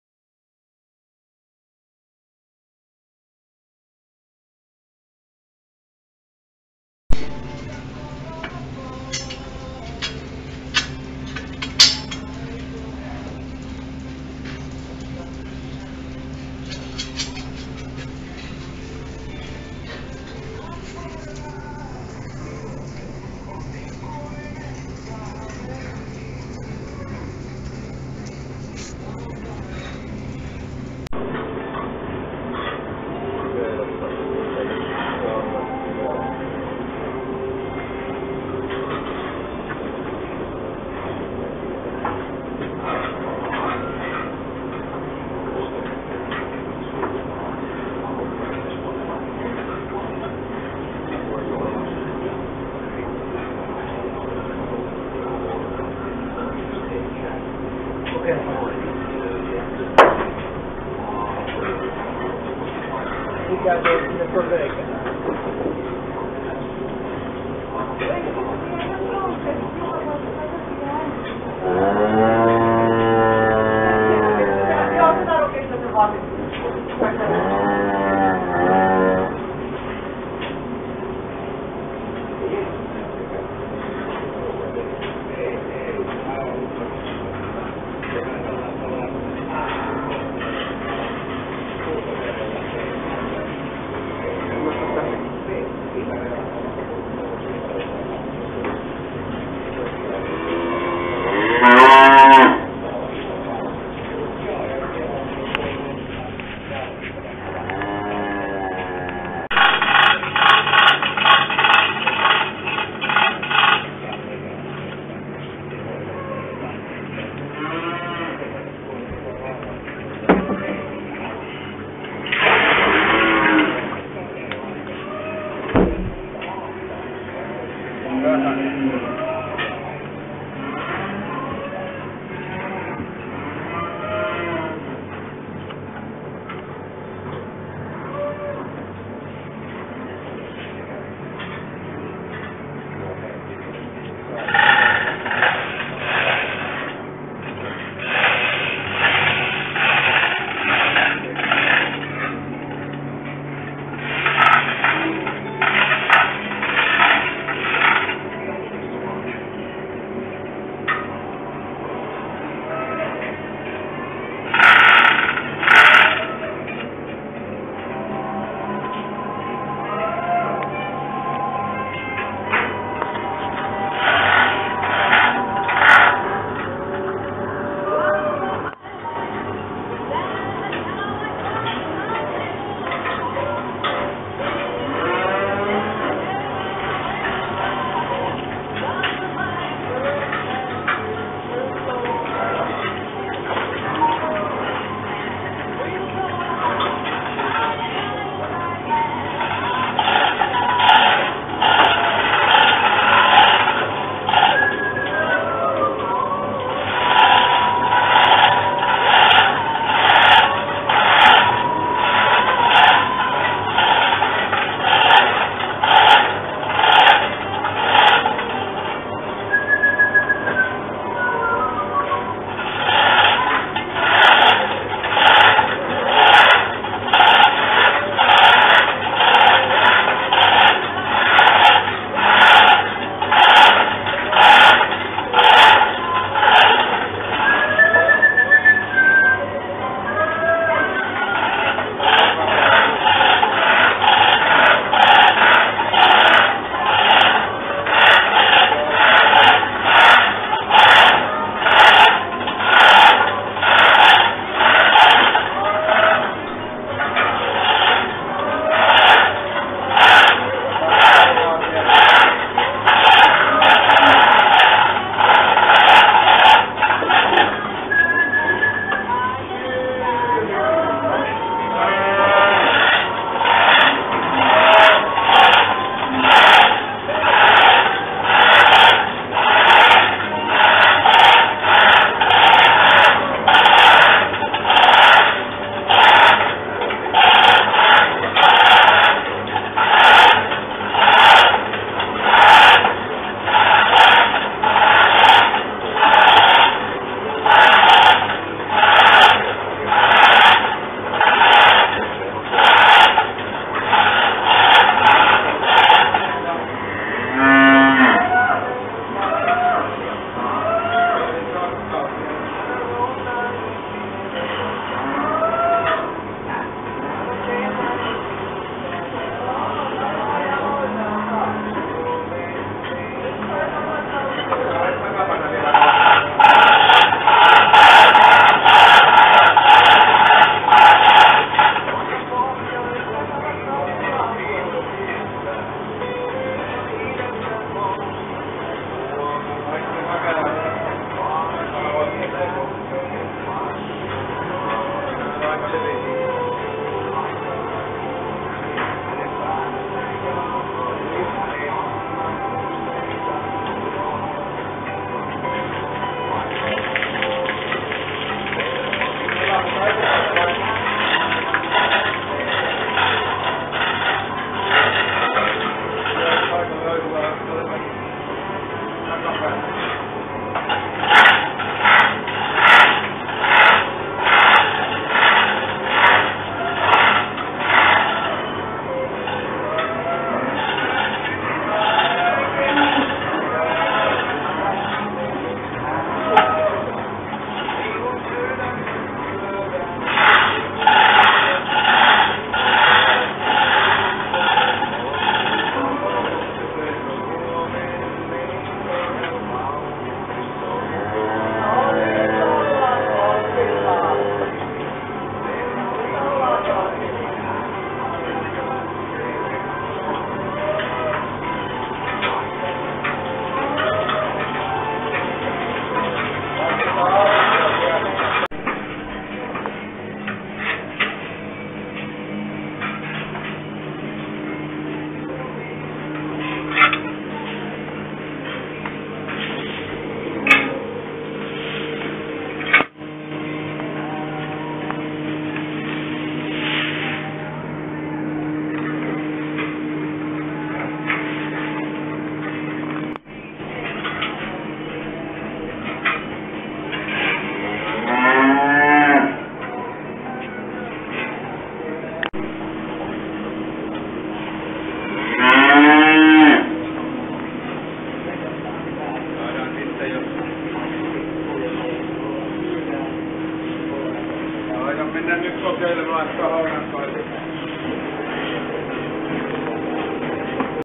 Lehmän nosto
Edellisenä päivänä Uguri-lehmä oli saanut poikimahalvauksen ennen poikimistaan. Eläinlääkäri kävi antamassa sille kalkkia suoneen. Yöllä Uguri sitten poiki mutta valitettavasti vasu menetettiin pitkittyneen poikimisen takia. Tänä aamuna Uguri söi ja joi mutta ei aivan jaksanut nousta ylös jaloilleen. Lehmän ylös nostamiseksi meillä on tällainen teline millä eläin saadaan jaloilleen.. Nostettaessa lehmää se voi näyttää melko kurjalta mutta voin vakuuttaa ettei siihen satu.